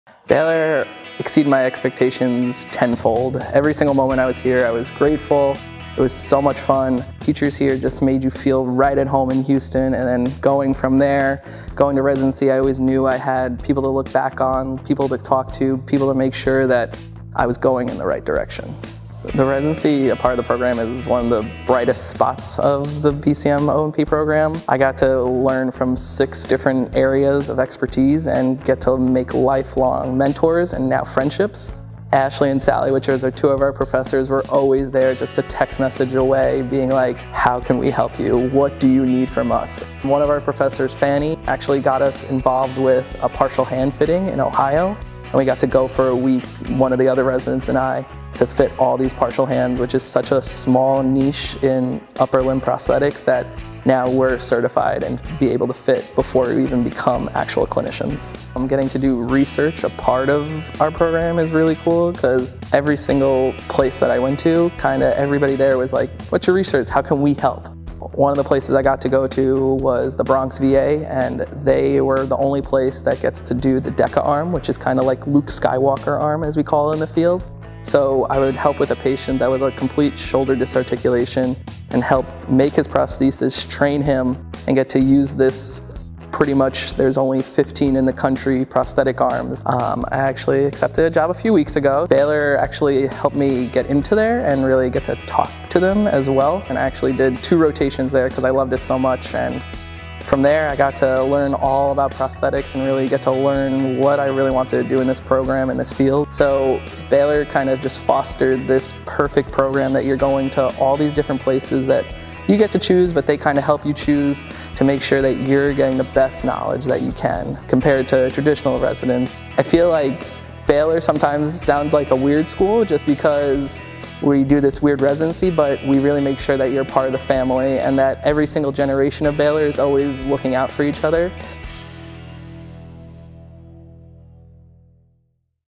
Student Voices: Edward Krische
Edward Krische, a student in the Orthotics and Prosthetics Program in the School of Health Professions, shares his Baylor College of Medicine experience.